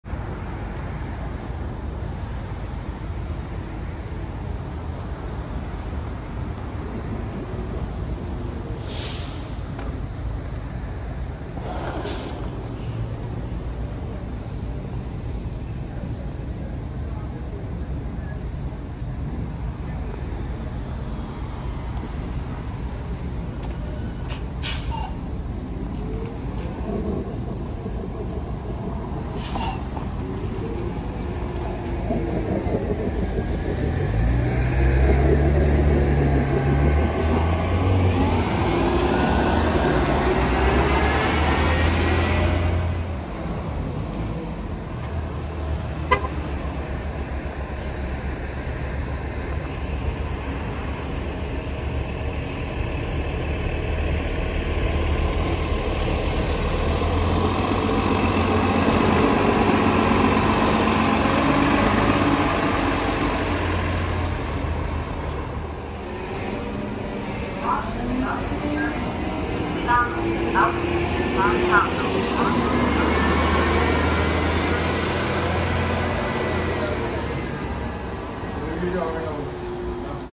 Montgomery County Transit Ride On Action at Germantown Transit Center #1
Happy Labor Day, This is a short compilation of Montgomery County Transit Ride On buses at Germantown Transit Center.

Taken: 8/31/21